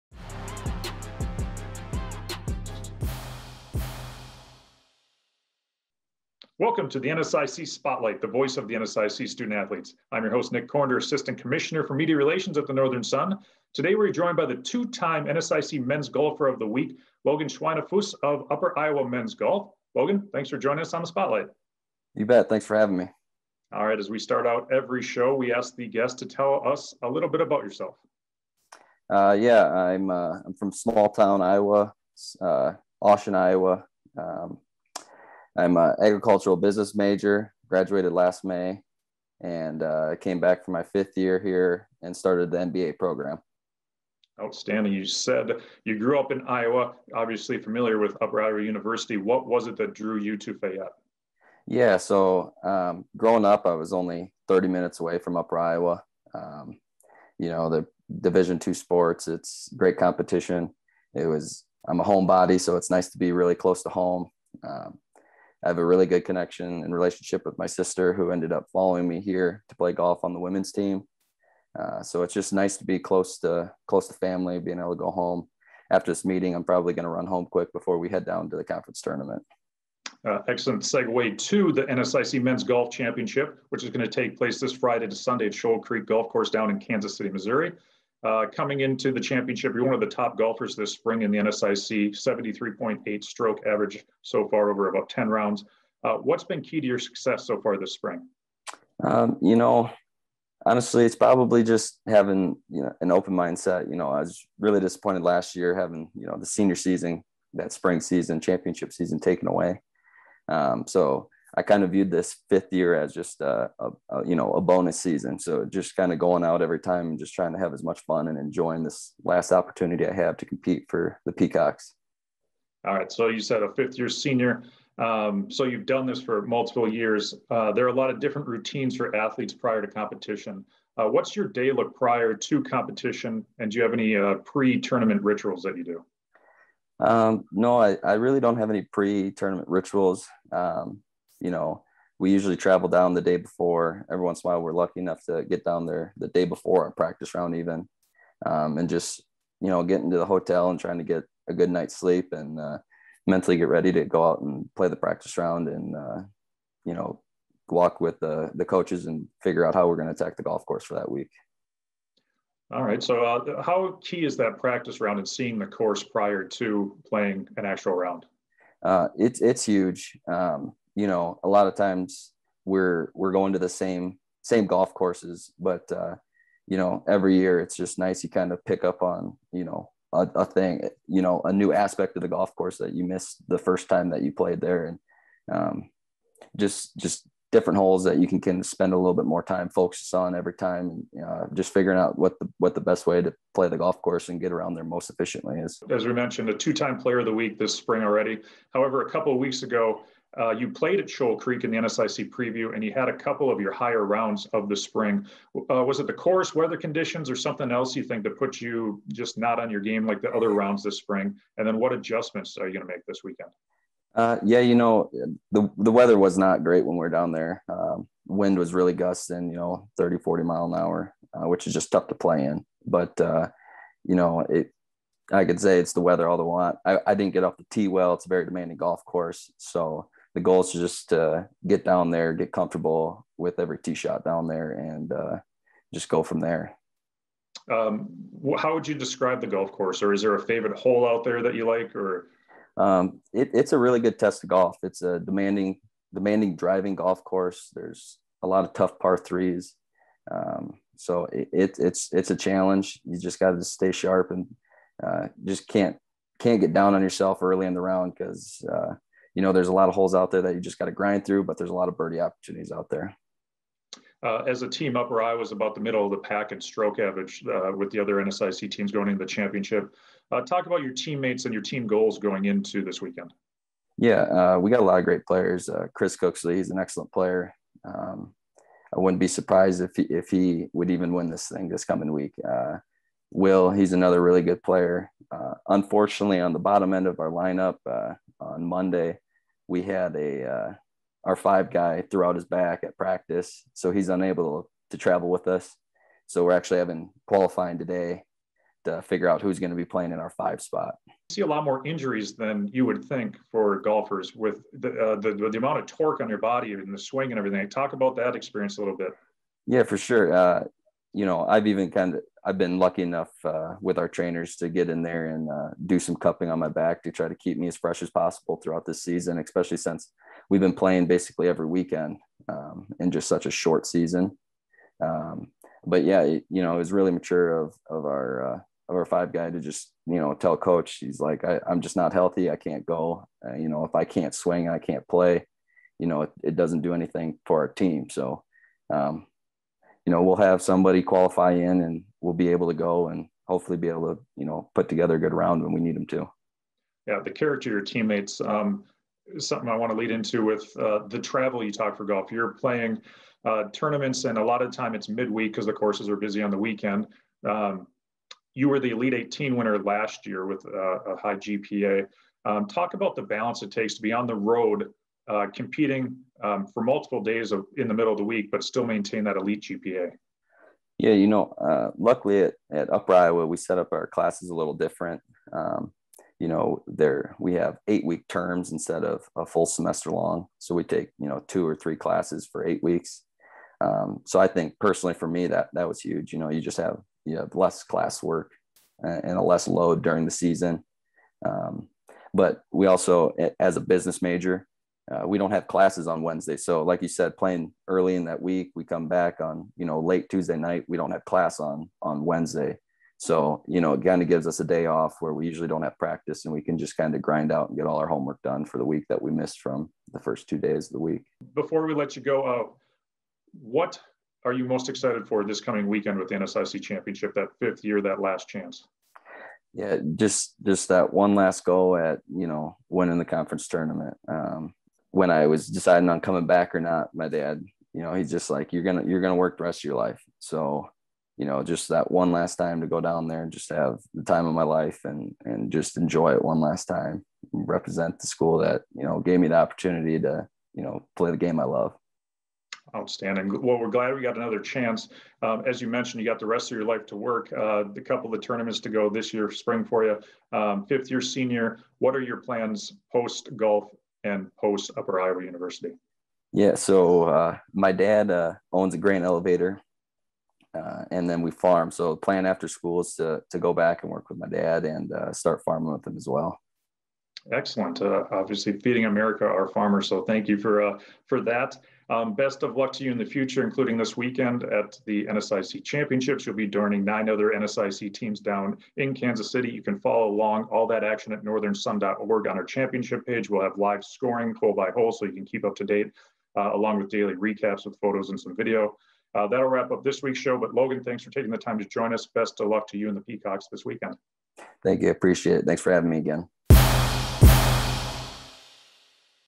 2020-21 NSIC SPotlight Ep. 14 - Logan Schweinefus - Upper Iowa
Episode 14 of the 2020-21 NSIC Spotlight features Upper Iowa senior golf Logan Schweinefus.